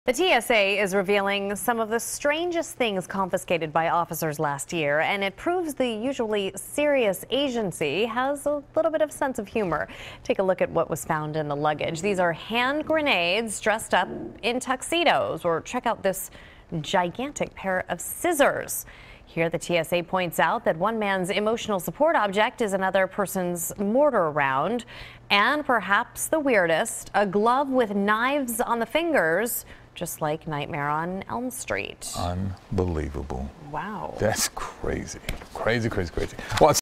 TSA Reveals Weirdest Things Confiscated Last Year
Take a look at what was found in luggage.